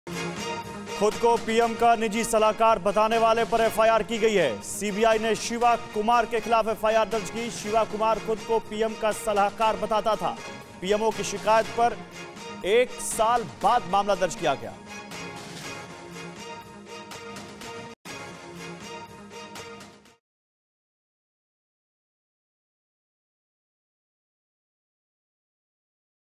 Shiva Kumar खुद को बताते थे PM का निजी सलाहकार, CBI ने दर्ज की FIR
खुद को पीएम का निजी सलाहकार बताने वाले पर FIR, CBI ने शिवा कुमार के खिलाफ दर्ज की FIR, शिवा कुमार खुद को पीएम का सलाहकार बताता था.
FIR against someone who claims to be PM's personal advisor, CBI has filed FIR against Shiva Kumar, Shiva Kumar used to call himself PM's advisor.